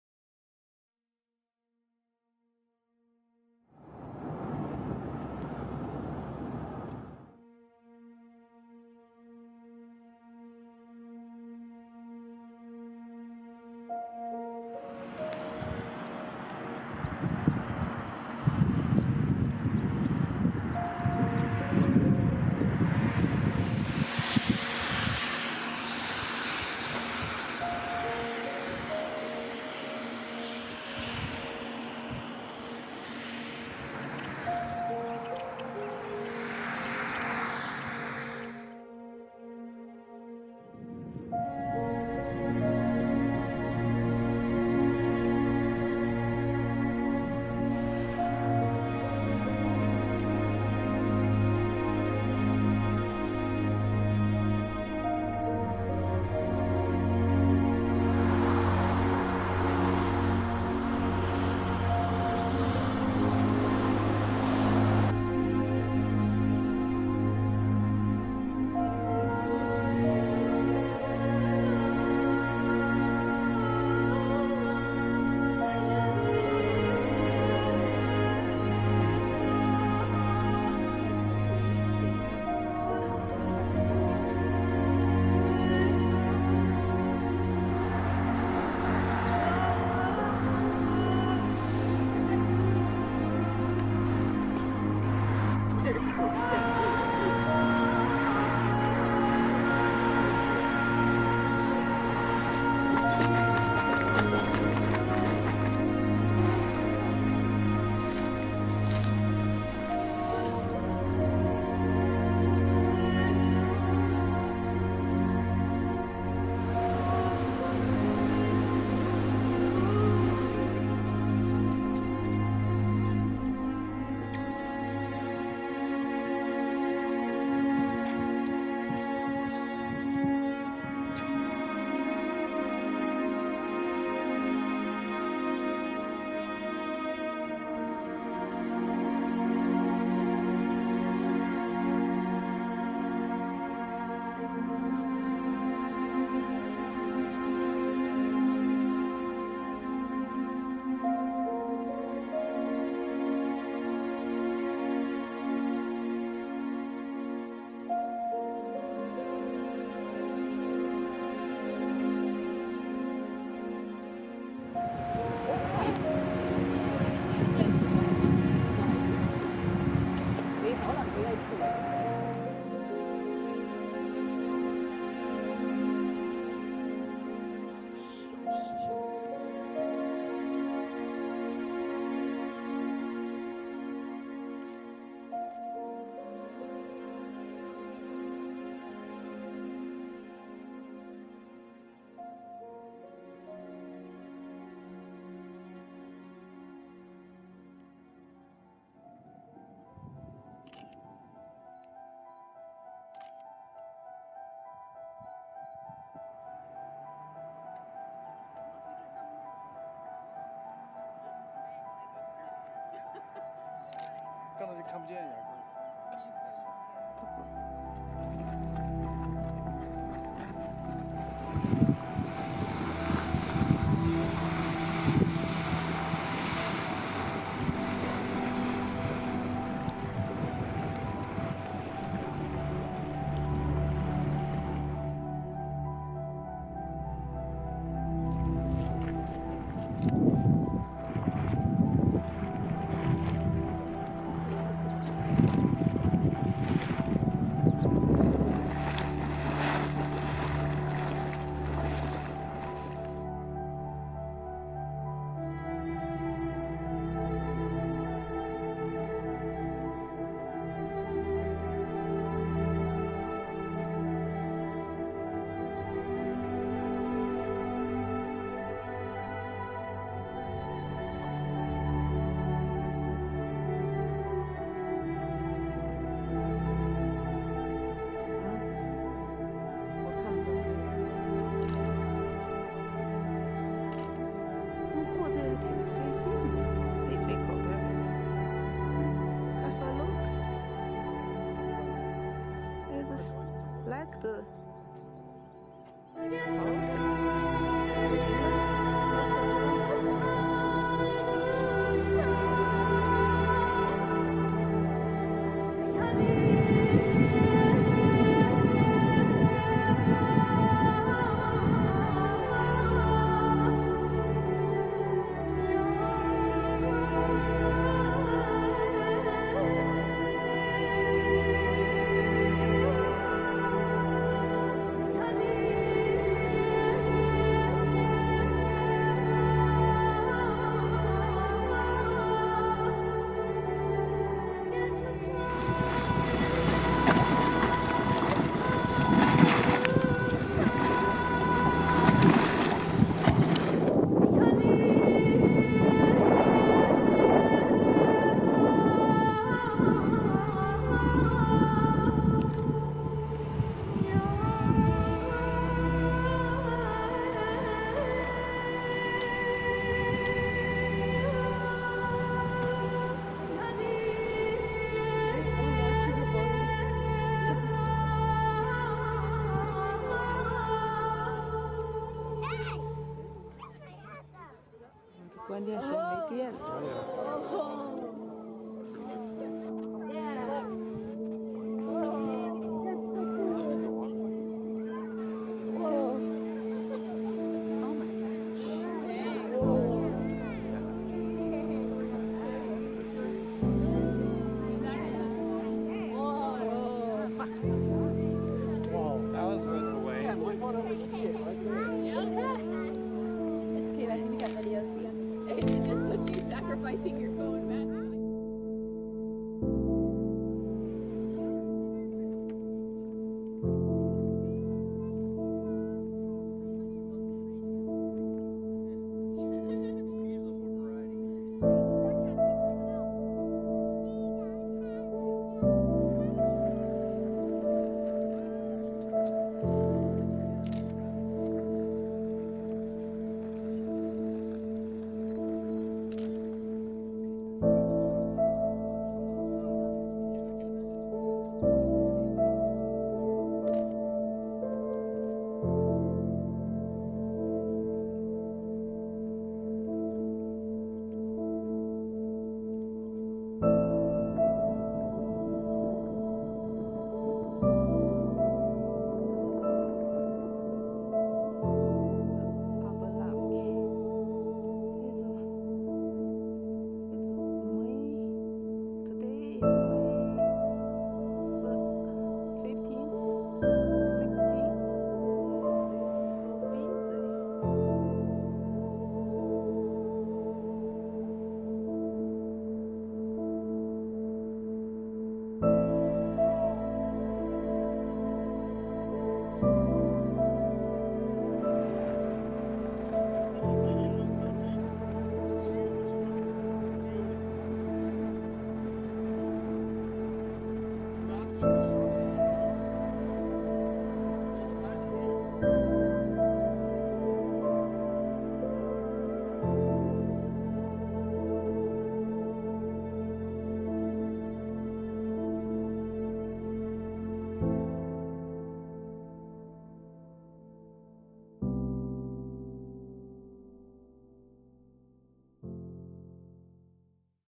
Visit to Yellowstone National Park - US - 美国黄石国家公园
Yellowstone National Park is a like a magnet that alway attracts me with her magnificent beauty and natural wonders. This is our second visit to the park after the first visit about 14 years back. We went into the park from the west and exited from the east this time, a wonderful experience again. We will perhaps go back to park again for the third visit in the near future.